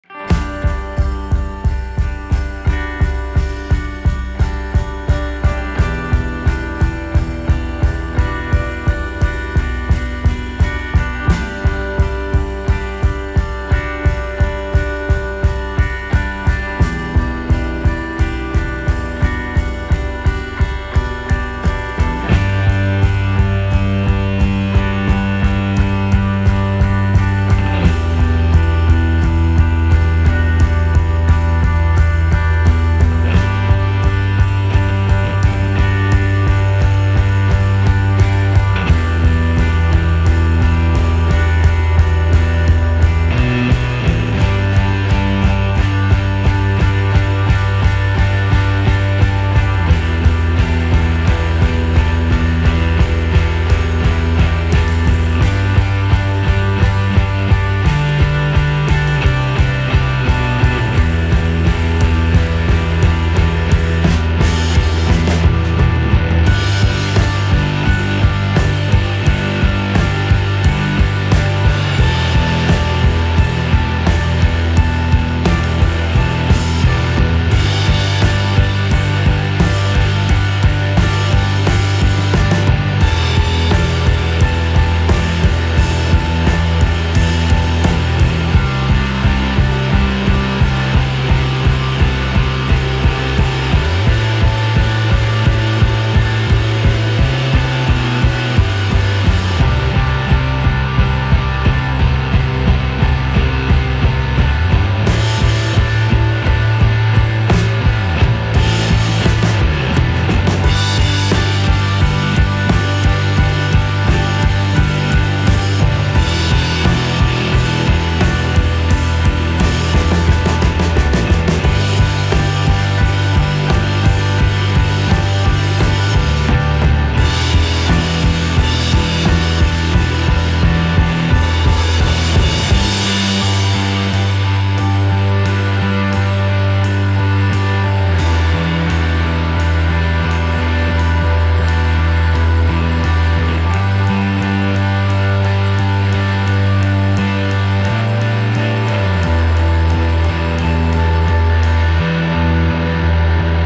Kona Cinder Cone 1996 @ Rutland Water 2015 HD gopro
Short section of the Rutland Water circuit, UK